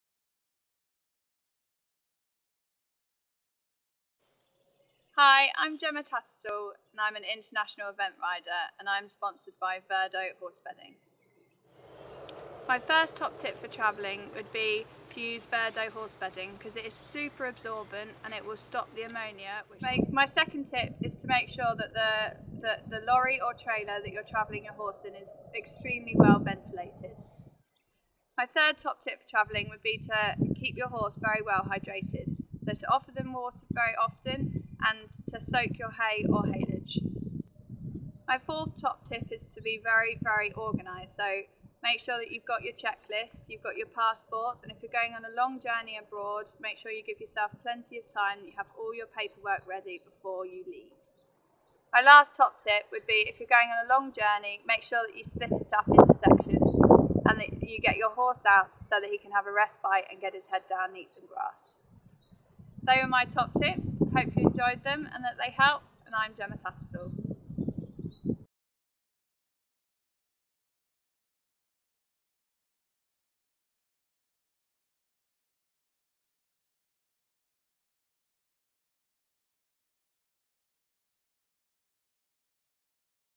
Gemma Tattersall's Top Travelling Tips
Verdo Horse Bedding sponsored rider international eventer Gemma Tattersall gives readers her top travelling tips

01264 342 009